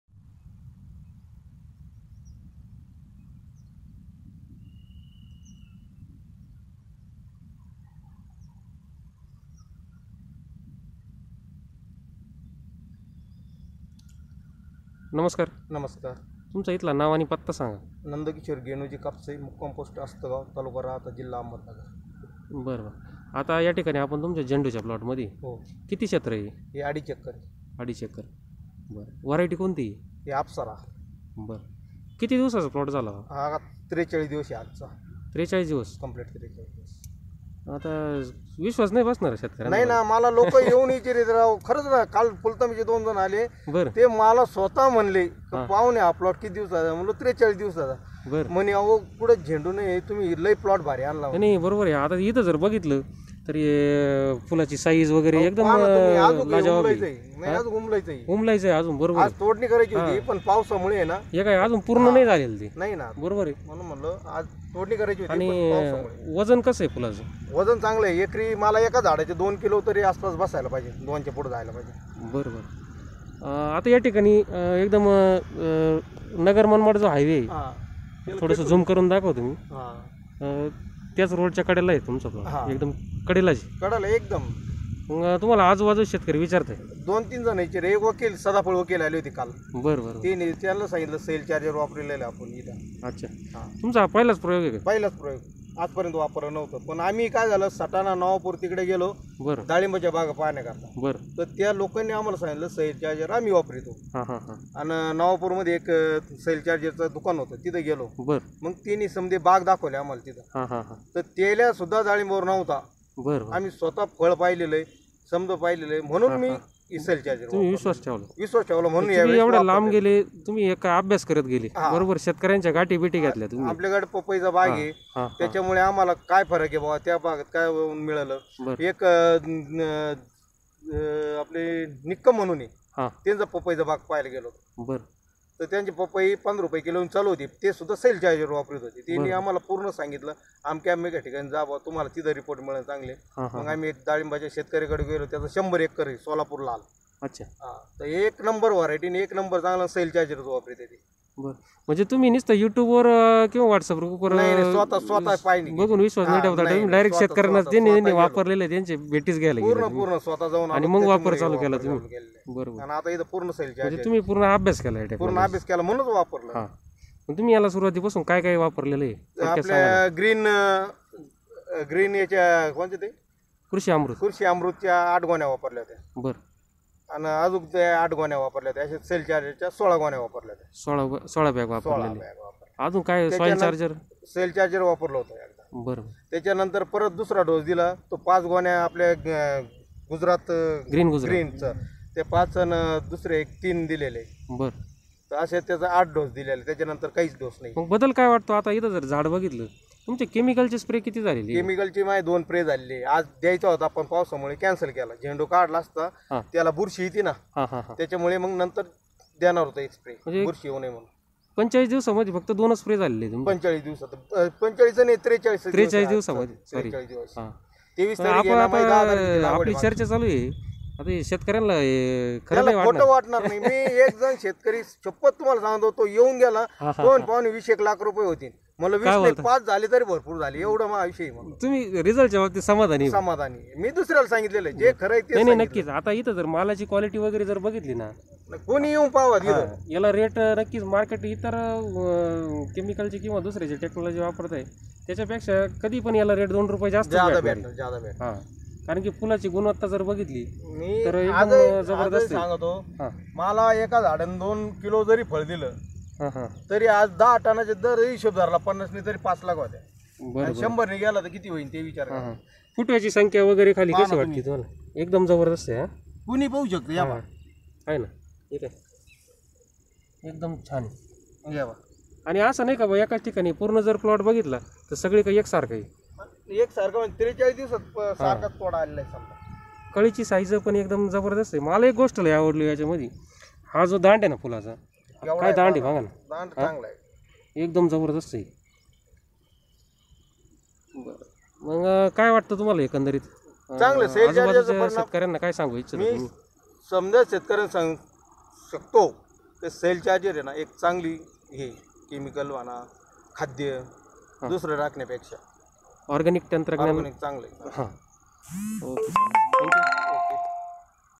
Soil charger tech. पावसाळ्याचा मार झेलून 43 दिवसांचा झेंडू पाहून शेतकरी चकीत का होतात ⁉️ SCT शेड्यूल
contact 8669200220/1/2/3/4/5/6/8